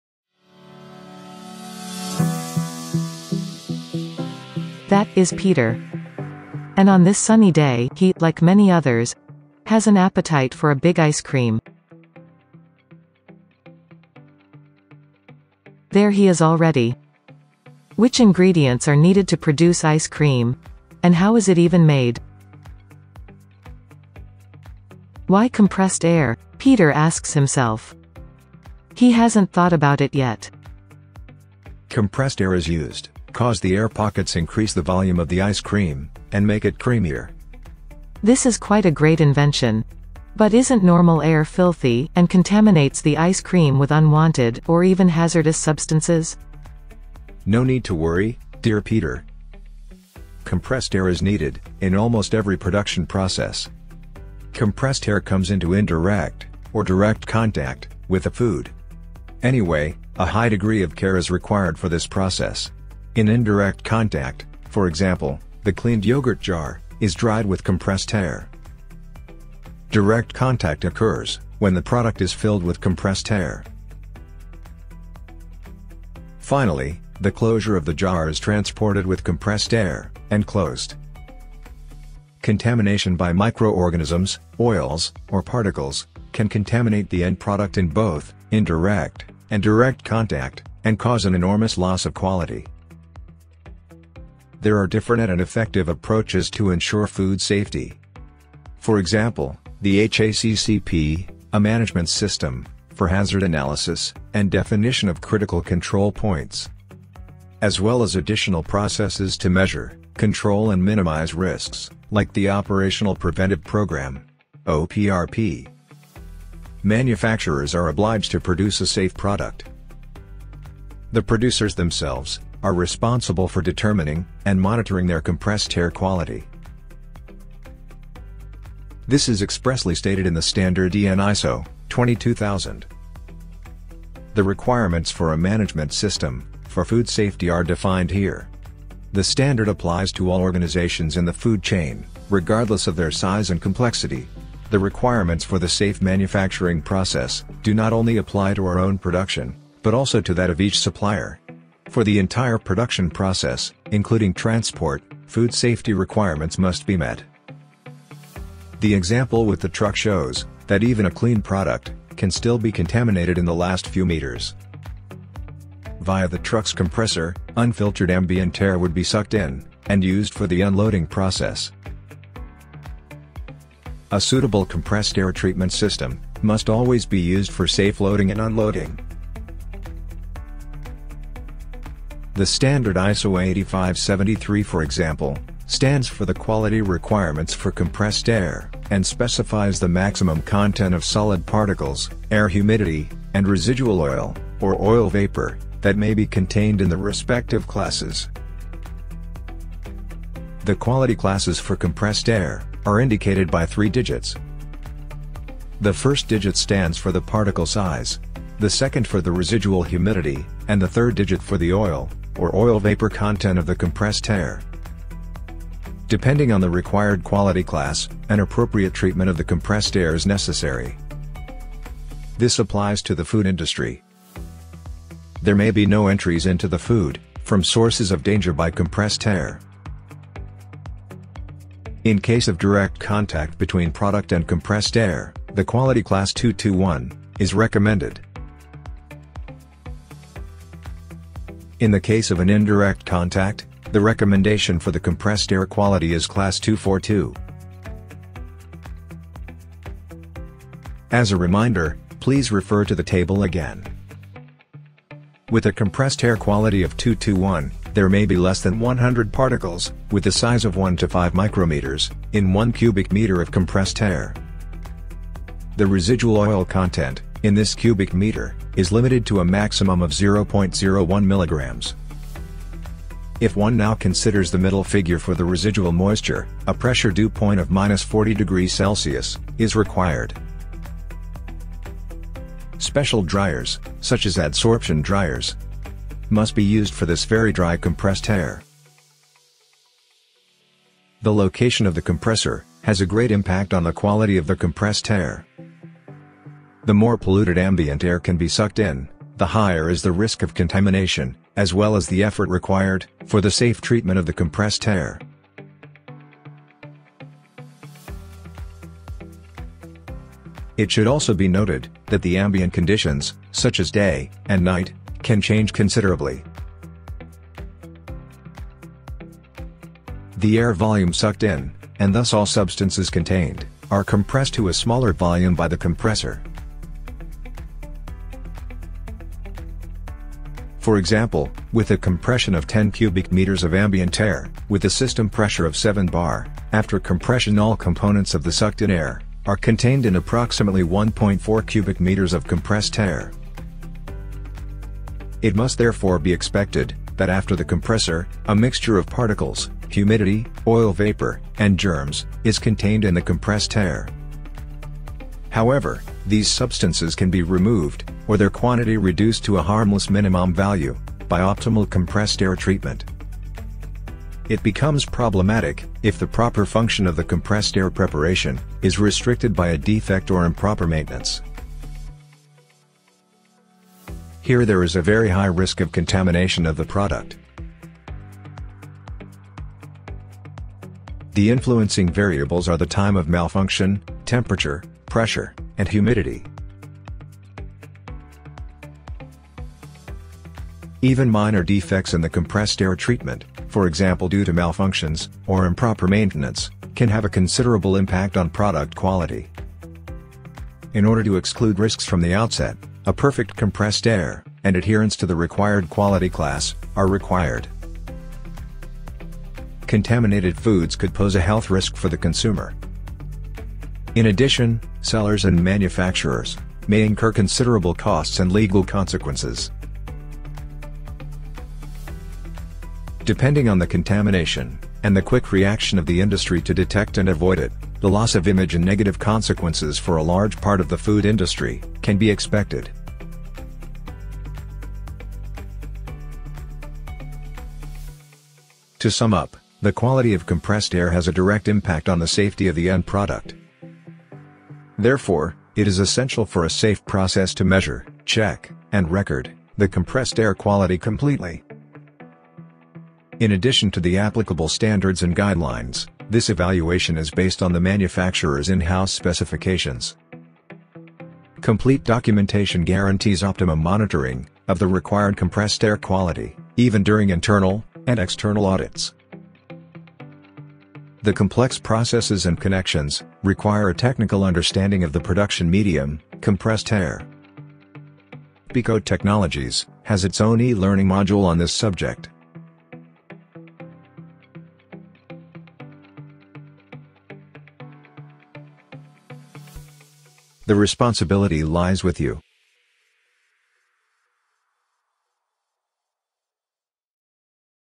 Using Compressed Air In The Food Industry
This video displays the vital importance and impact of compressed air quality when it comes to producing and processing food safely.

Read more about the food and beverage industry using compressed air